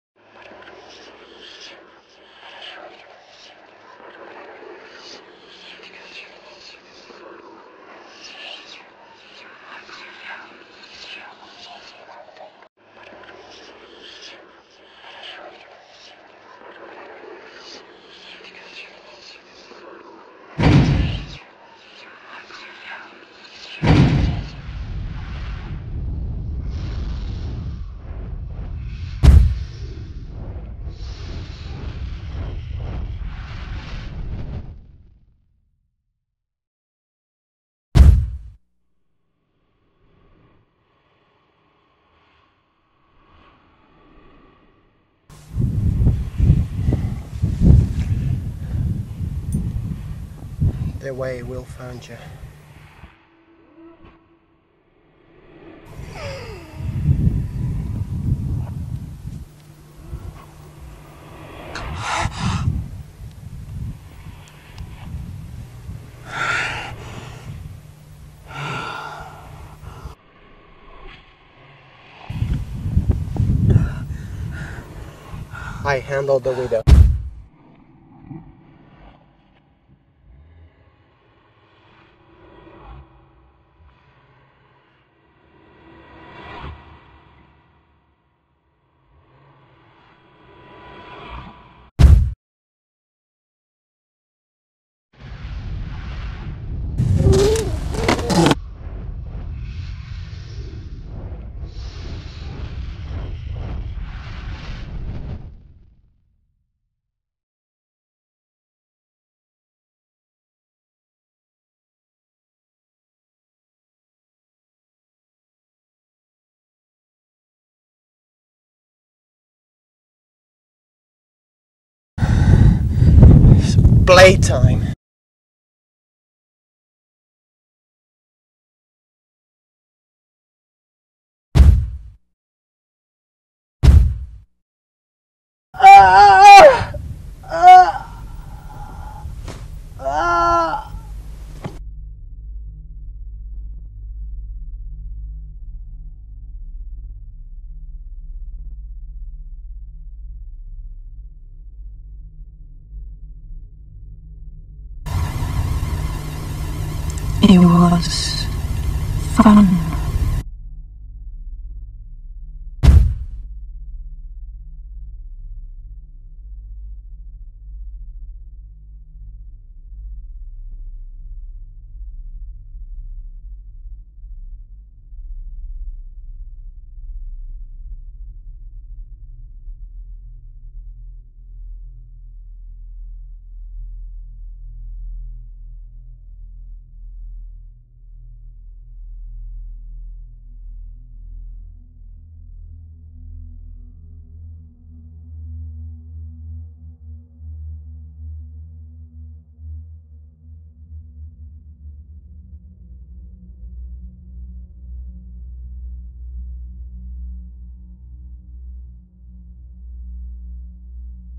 lost in the desert
Whats Lost can be found again. The man lost in the desert is found again by his friend who then dares to fight The Widow...